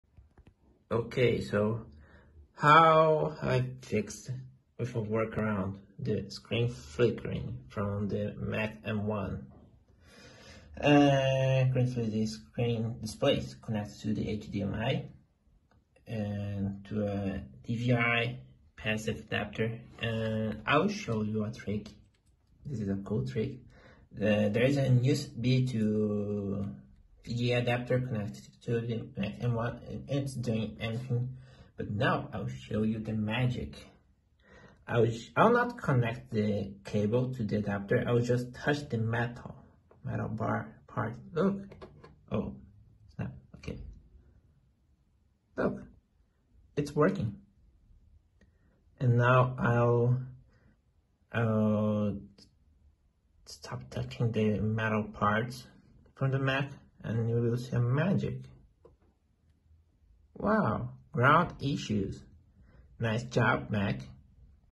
(Workaround) Apple Mac M1 Screen flickering
It seems like this Mac Mini M1 has some ground issues that affect my screen, to solve this, all I did is connect a VGA cable to the monitor and a USB to VGA adapter to the Mac Mini.
I didn't connect the VGA cable to the adapter, all I did was touch the metal parts of both adapter and VGA cable and suddenly the screen flickering stopped happening.

Yeah, I know it's dumb that I fixed my HDMI output with a VGA cable.